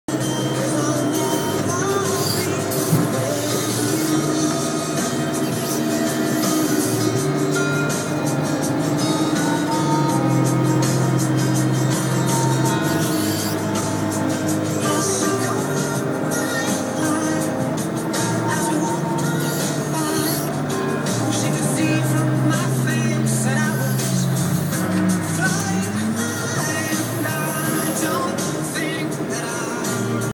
Sgombero neve Nella Martino
Sgombero neve professionale, rampe garage, piazzali condomini con ddieci 26.6 veramente eccezionale per certi lavori.
Si entra nei garage, non si impianta mai, si passa sotto I poggioli, si caricano camion da dietro nei vicoli stretti ecc. Farò altri video per farvi vedere cosa puó fare.